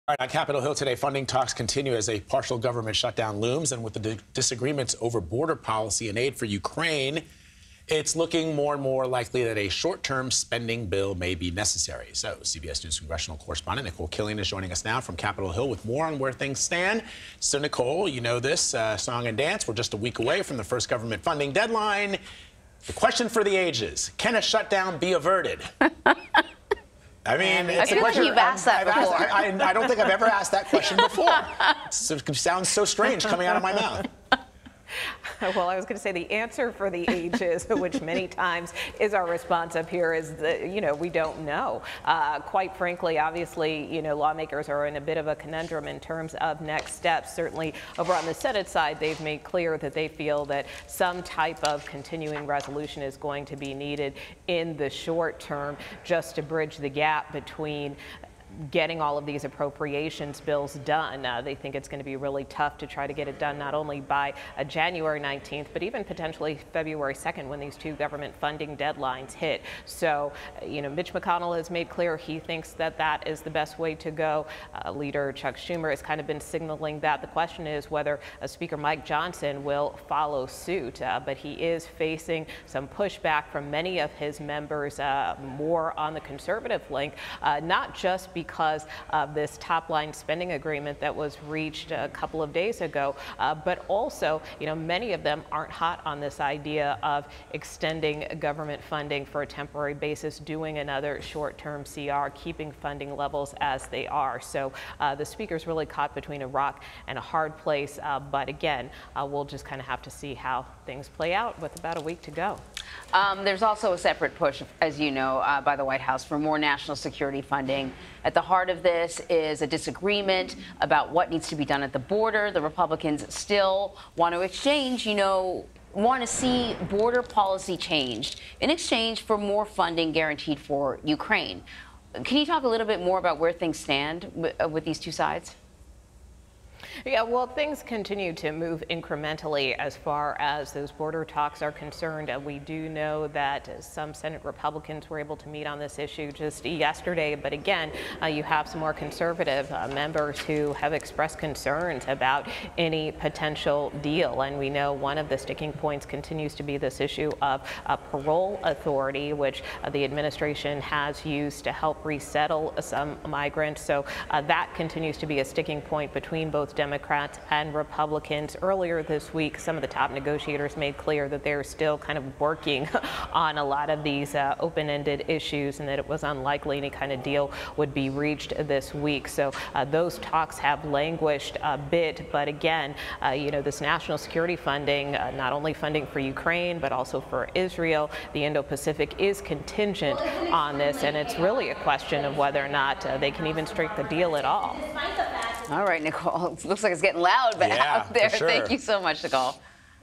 Short-term spending bill likely needed to avoid partial government shutdown
Funding talks continue on Capitol Hill with the deadline to avert a partial government shutdown nearly a week away, but disagreements remain over border policy and aid for Ukraine. CBS News congressional correspondent Nikole Killion has more.